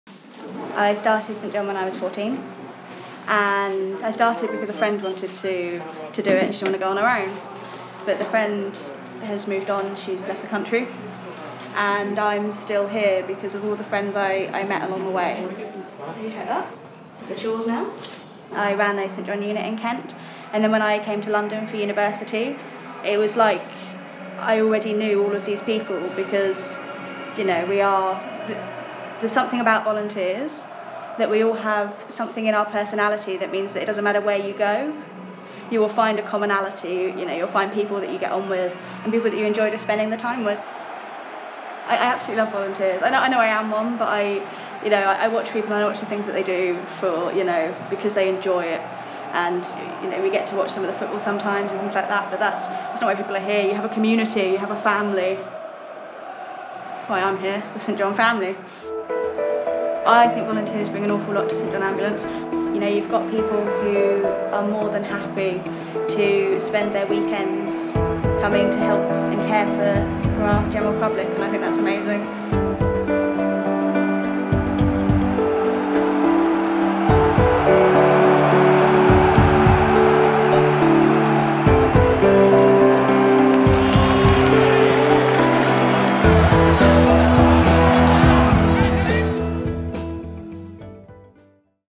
London Stadium - Ask Me Why I love volunteering with St John
Danielle has been with St John Ambulance since the age of 14. She shares why volunteering and the St John family are so important to her whilst on duty at London Stadium.